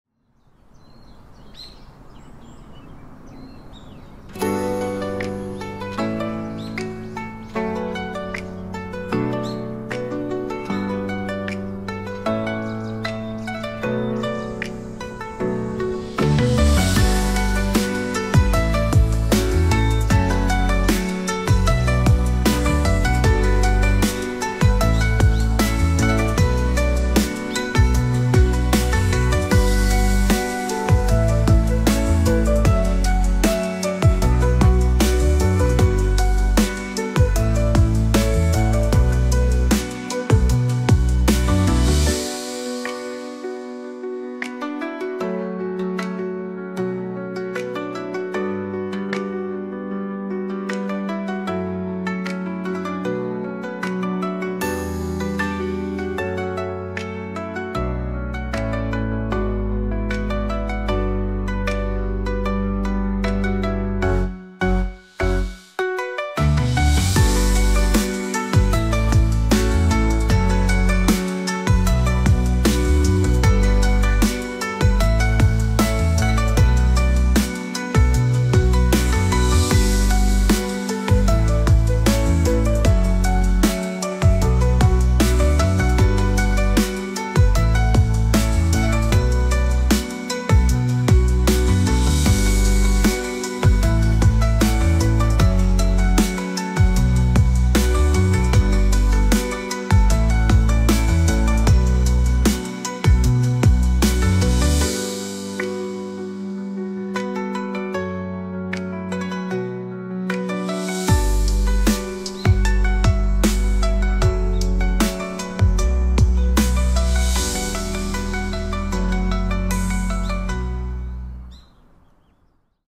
My Garden Rose Cut Flowers🌹Soothing Time Lapse
May my roses bring you peace and joy. Rose varieties shown: Seraphim, Roald Dahl, Cherry Parfait, Brindabella Empress, Sunbelt Savannah, ...